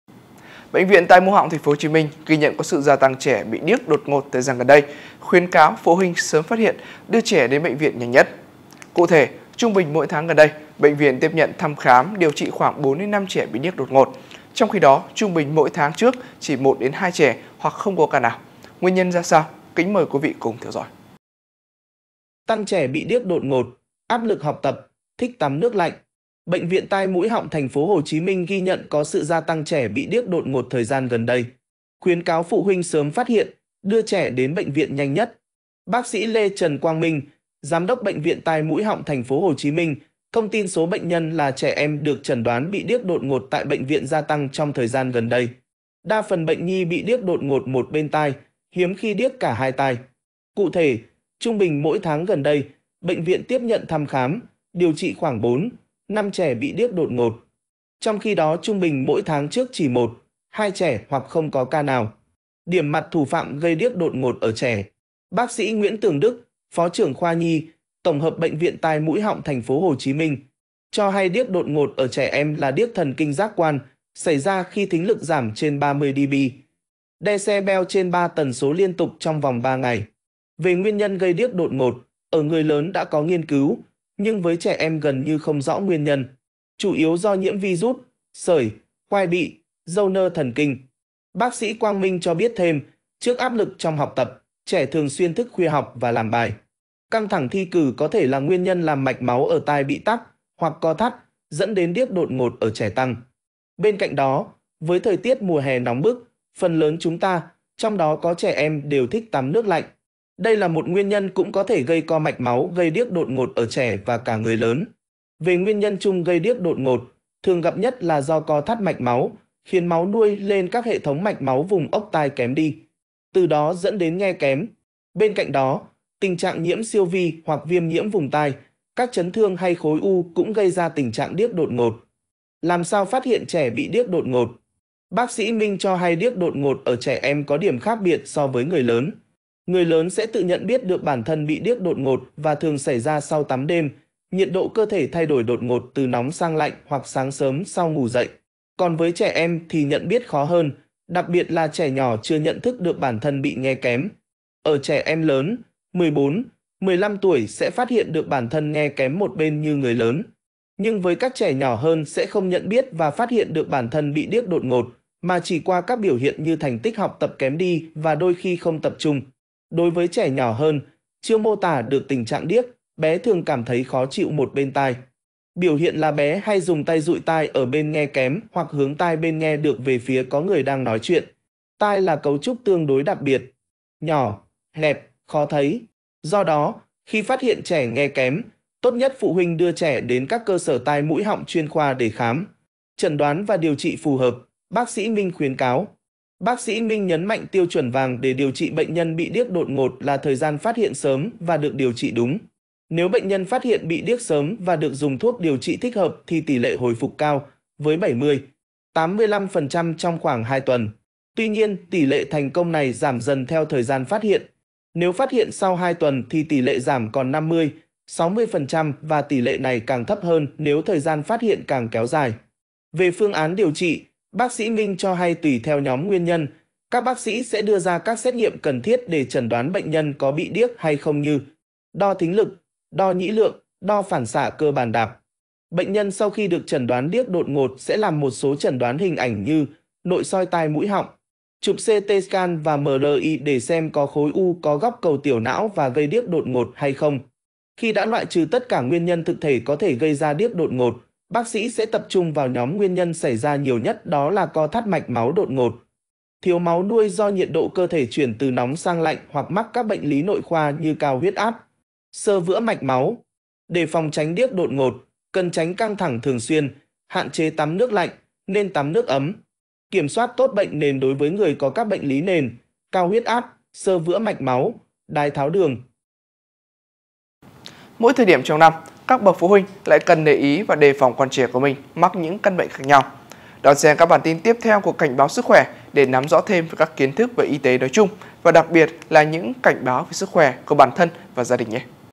Cảnh báo sức khoẻ | Tăng trẻ bị điếc đột ngột Áp lực học tập, thích tắm nước lạnh
► Nội dung chính: Cảnh báo sức khoẻ | Tăng trẻ bị điếc đột ngột Áp lực học tập, thích tắm nước lạnh